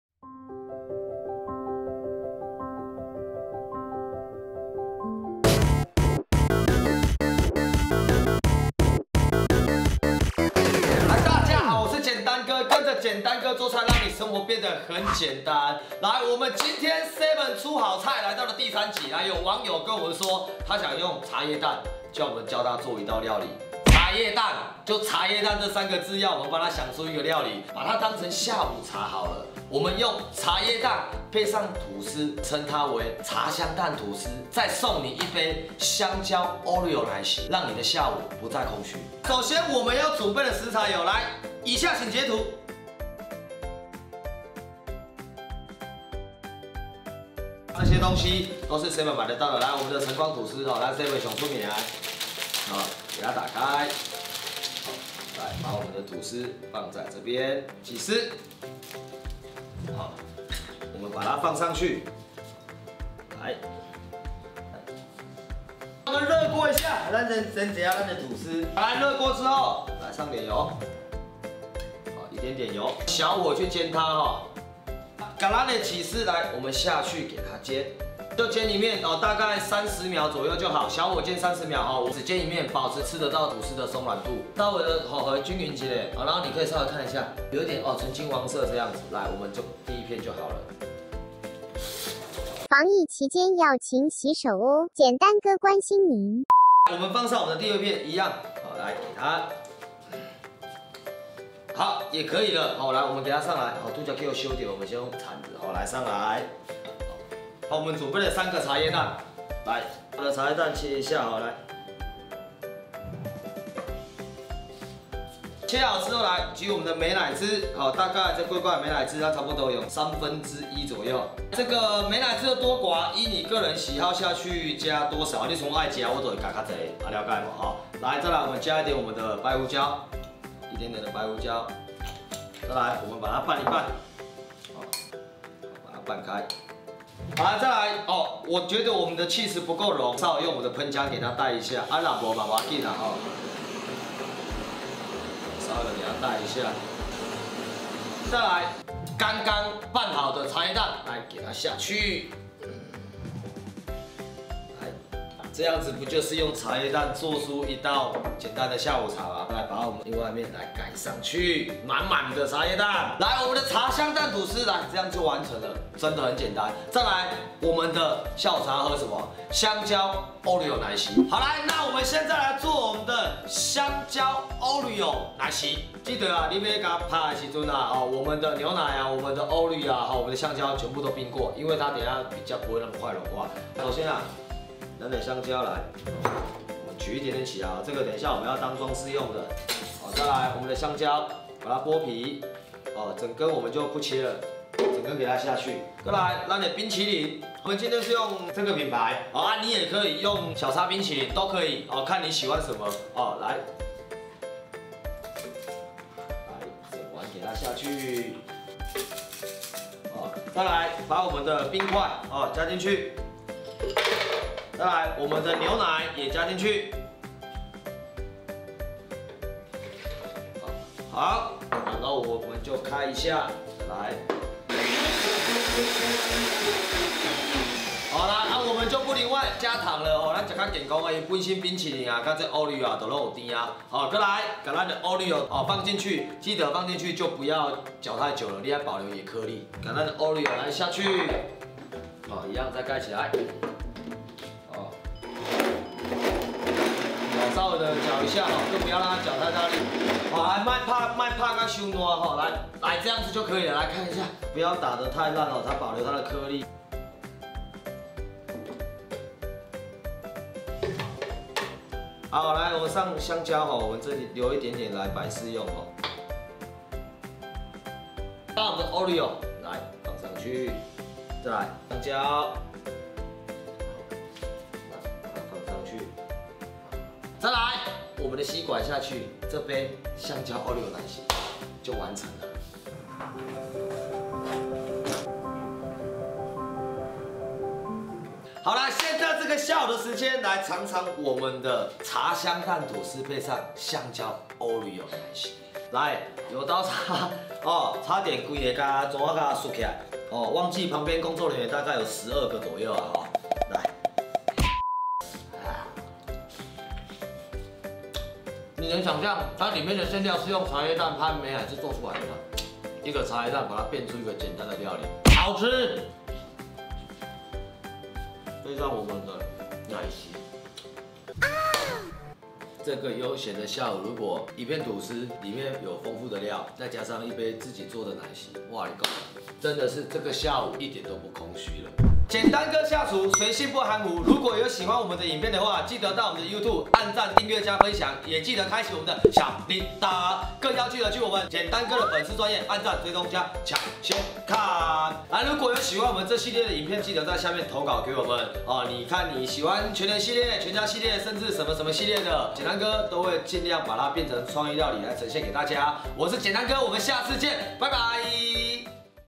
我就胖！超商下午茶雙拼【茶香蛋土司/香蕉Oreo奶昔】
大家好我是簡單哥！
這次跟大家分享
在超商就可以購買到的食材
回家就能輕鬆做出簡單下午茶

🍴茶香蛋土司 需要準備的食材🍴
     👉茶葉蛋    3顆
     👉沙拉醬    1條
     👉吐司        2片
     👉起士片    3片
     👉胡椒      少許

🍴香蕉Oreo奶昔 要準備的食材🍴

     👉香蕉           1根
     👉OREO        7片
     👉牛奶      120.c.c
     👉冰塊        150克
     👉冰淇淋      適量
     👉水         100.c.c

🥘茶香蛋土司開始簡單製作啦🥘
①起士片對半
②先把吐司單面煎一下，未煎過的一面鋪上起士
③將茶葉蛋去殼一切八
④放入碗加入美乃滋、鹽、胡椒攪拌後備用
⑤把配料鋪上另一片吐司上
⑥再把另外一片吐司鋪上有蛋的吐司上

🥘香蕉Oreo奶昔 開始簡單製作啦🥘
①香蕉去皮切片留1/3備用
②把牛奶、香蕉、冰淇淋放進去攪拌機攪拌一下
③加入冰塊攪拌至冰沙後加入OREO攪拌10秒

📢做菜跟著簡單哥 賴蛤蟆也能吃天鵝